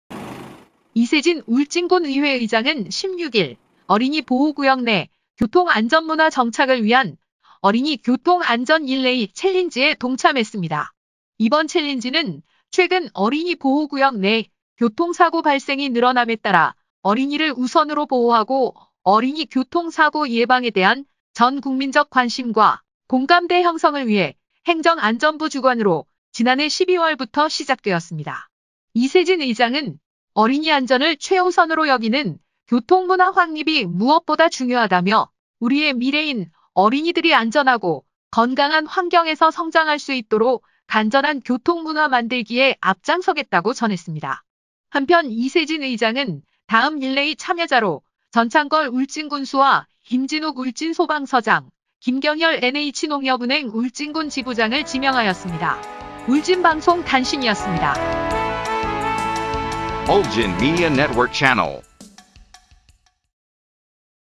[울진방송] 20210216(화) 이세진 울진군의장, ‘어린이 교통안전 릴레이 챌린지’ 동참
감사합니다.

(기자)
이세진 울진군의회 의장은 16일 어린이 보호구역 내  교통안전
문화 정착을 위한 ‘어린이 교통안전 릴레이 챌린지’에 동참했습니다.

 이번 챌린지는 최근 어린이 보호구역 내 교통사고 발생이
늘어남에 따라 어린이를 우선으로 보호하고 어린이 교통사고
예방에 대한 전국민적 관심과 공감대 형성을 위해
행정안전부 주관으로 지난해 12월부터 시작되었습니다.

 이세진 의장은 “어린이 안전을 최우선으로 여기는 교통문화
확립이 무엇보다 중요하다”며 “우리의 미래인 어린이들이
안전하고 건강한 환경에서 성장할 수 있도록 안전한
교통문화 만들기에 앞장서겠다”고 전했습니다.

 한편 이세진 의장은 다음 릴레이 참여자로 전찬걸 울진군수,
김진욱 울진소방서장, 김경열 NH농협은행 울진군지부장을 지명하였습니다.